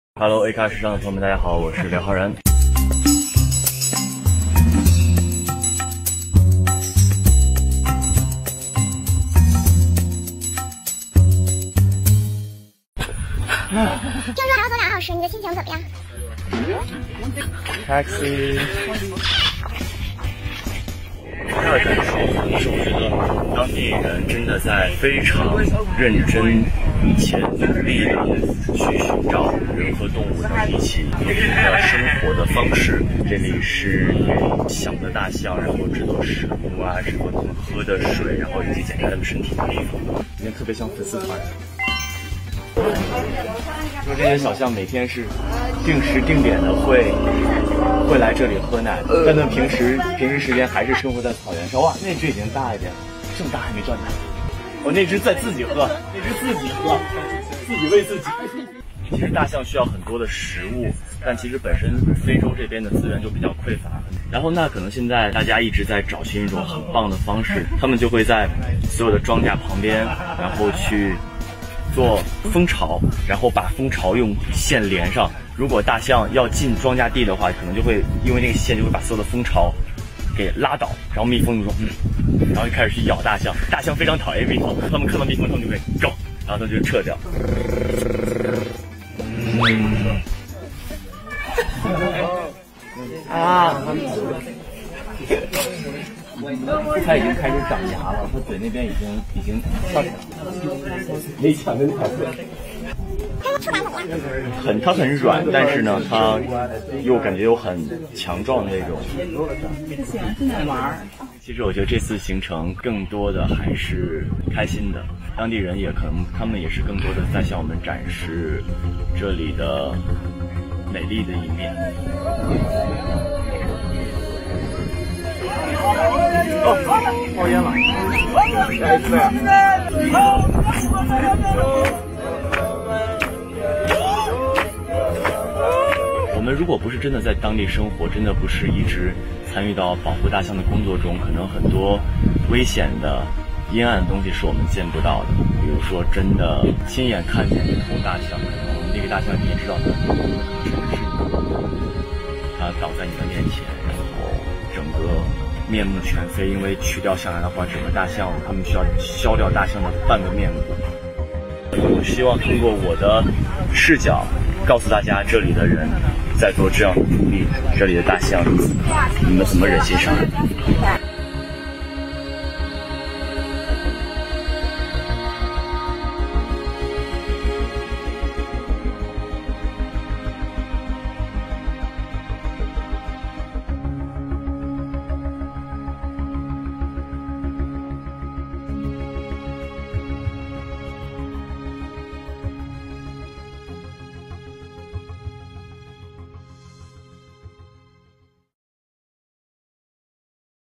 [Eng Sub] 3.18.19 Liu Hao Ran 刘昊然 Tiffany & Co. Kenya Trip Day 3
Day 3 of Liu Haoran's trip to Kenya with Tiffany & Co.'s Save the Wild Campaign. In which he fanboys over baby elephants, talks about his respect to the locals for their efforts in helping the elephants, and the brutal reality of poaching.

For more translations and updates on LHR, follow us @windchaser97 on Twitter!